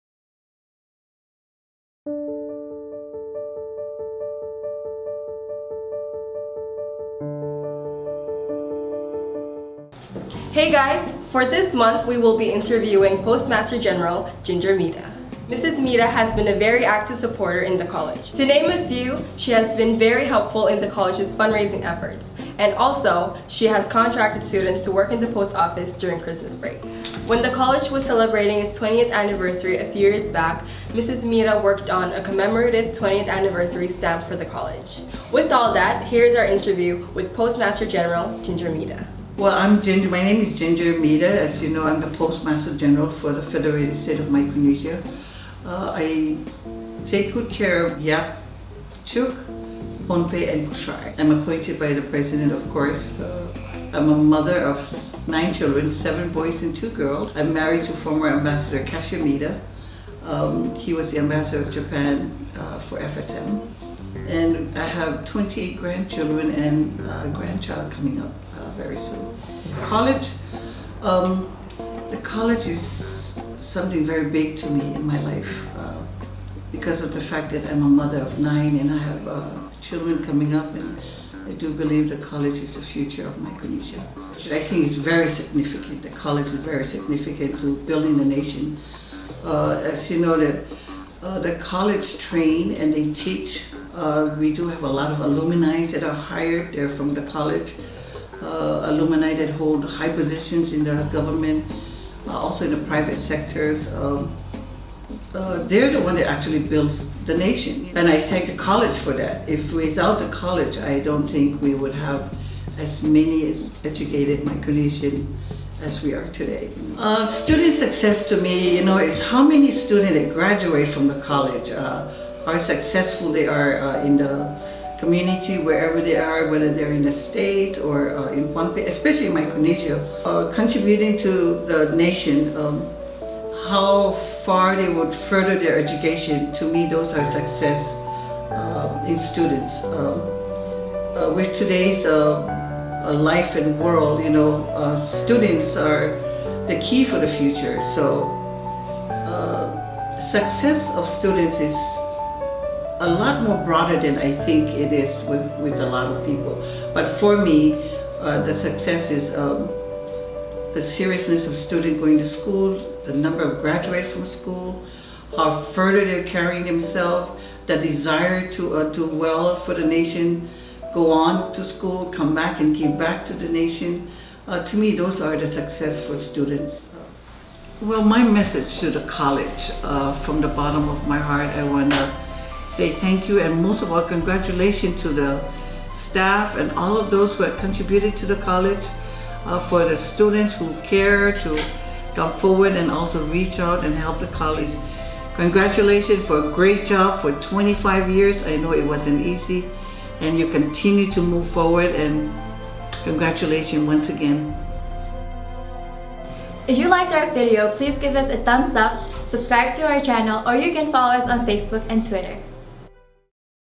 COM-FSM 25th: Ginger Mida
At the College of Micronesia-FSM student success is our success! A close friend of the college, Mrs. Ginger Mida , shares a message to the college community, and wishes the college the best as it will celebrate its 25th Anniversary in April 2018.